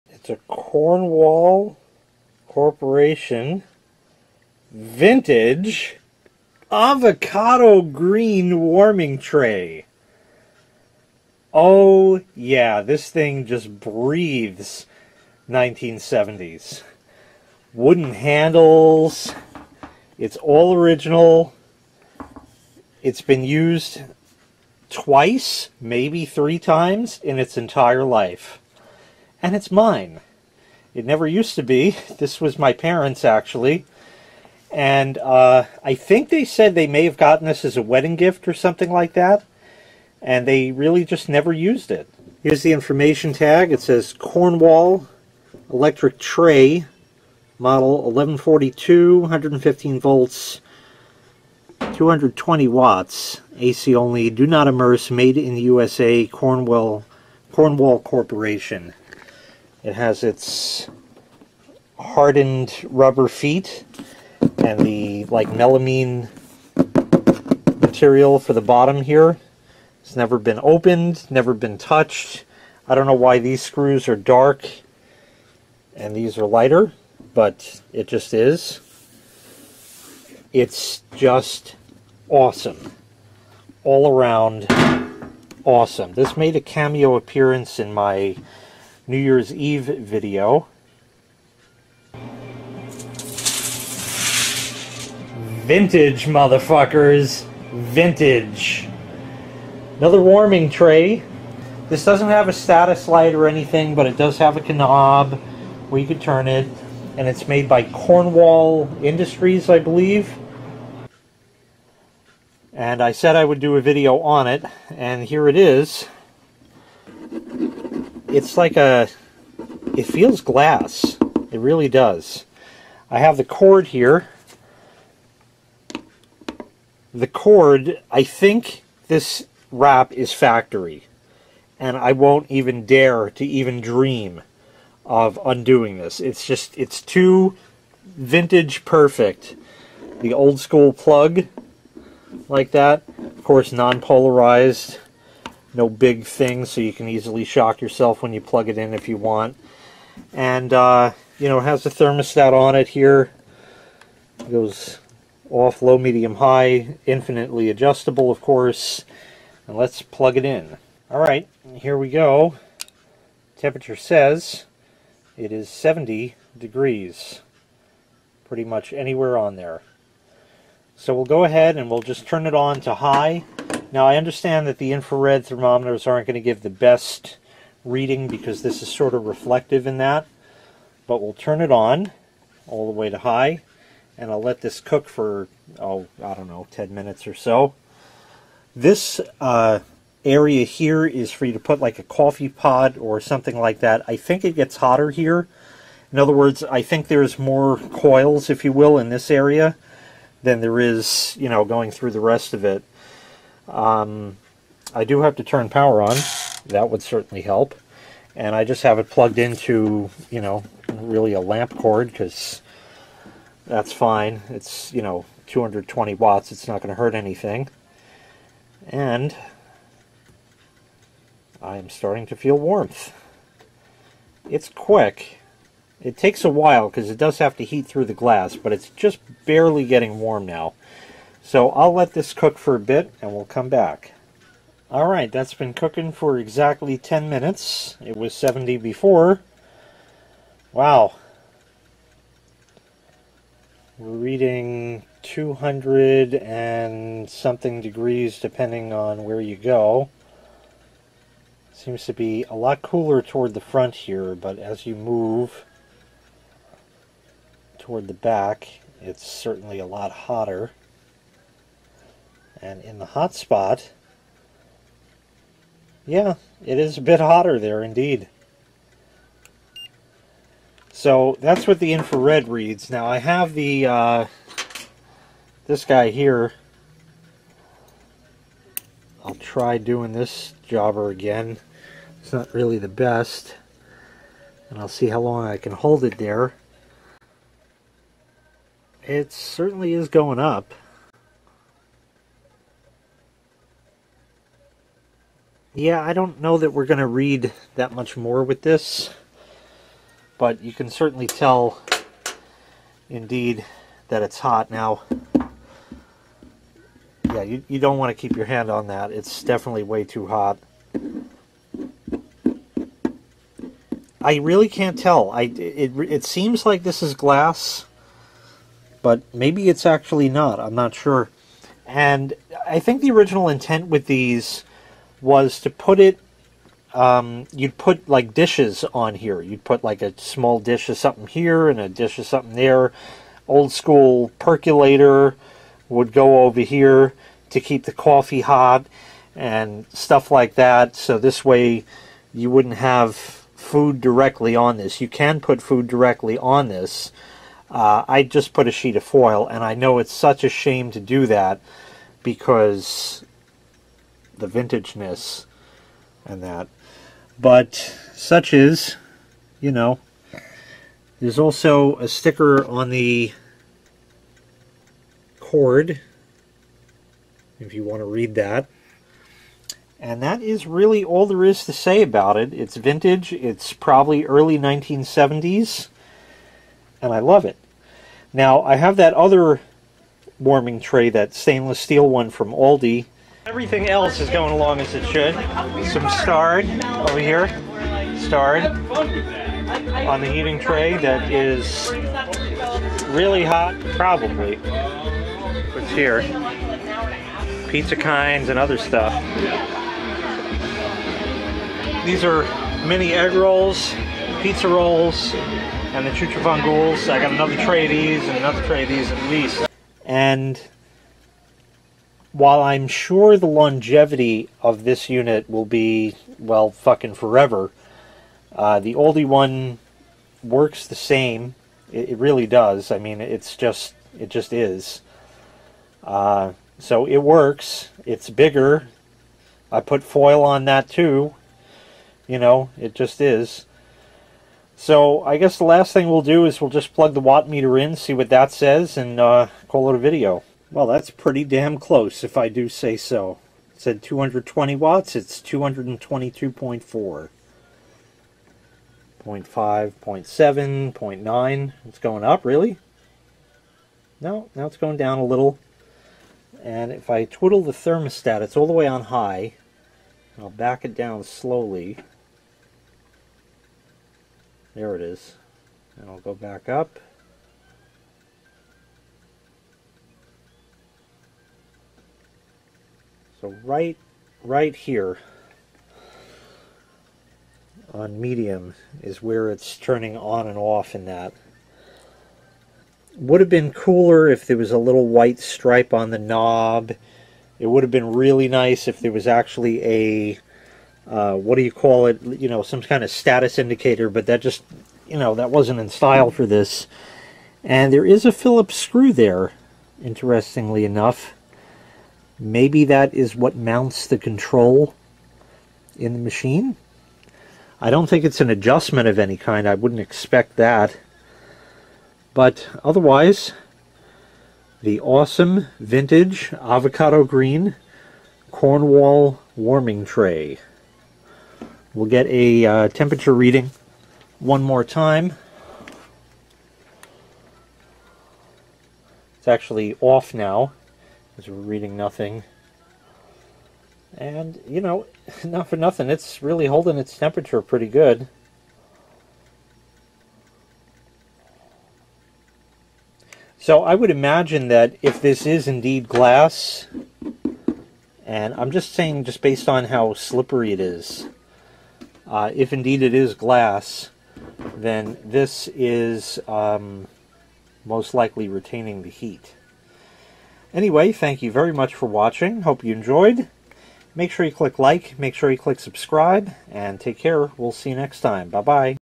VINTAGE 1970s Cornwall Electric Warming Tray, Model 1142
Originally recorded January 23, 2019.

This was my parents' warming tray.  It was used 2-3 times in its entire life.  They never used it, and decided to give it to me.  Chances are this happened because they went through an "archaeological dig" for something, and this turned up along the way.  Vintage?  Sure, I'll take it!

Truth be told, as much as I hate China pride stuff, the Aldi Kitchen Living warming tray that I have does just as good of a job, and it's larger.  Nowhere near as nicely styled of course, but that's the way it goes today, everything is plain stainless steel and boring.

I used this for the first time on New Year's Eve, and it worked a treat!